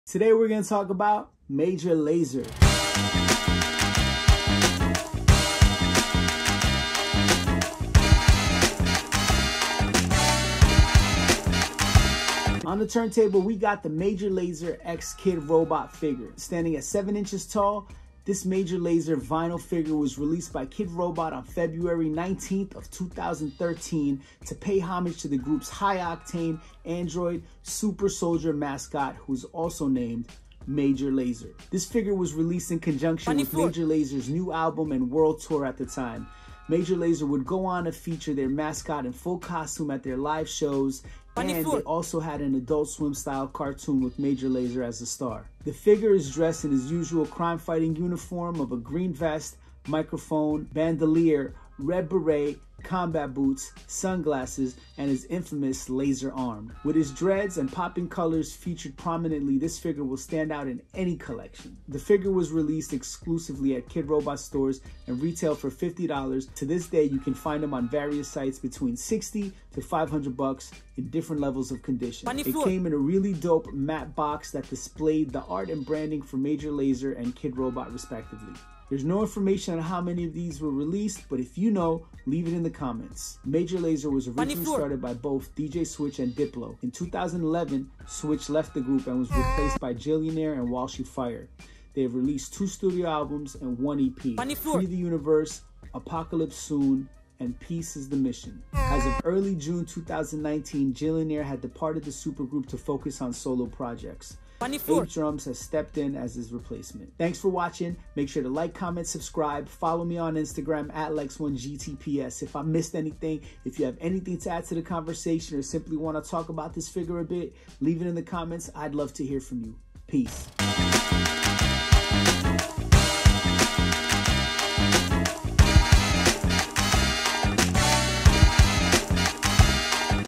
KidRobot Major Lazer Figure - No Hype Ep 156
Major Lazer x KidRobot
Back in 2013, KidRobot dropped a figure paying homage to one of the hottest EDM/Reggae group bubbling at the time, Major Lazer. In the height of the group’s popularity, KidRobot produced and released a 7 inch version of the group’s mascot Major Lazer. The character even got it’s own animated T.V. show on Fox even though it was originally created for Adult Swim. The show is available to stream on Hulu today.  The show featured voice work form John Boyege (Star Wars), Aziz Ansari, Trinidad James, Riff Raif and Andy Samberg just to name a view. All in all, anything Major Lazer is connected to is straight dope.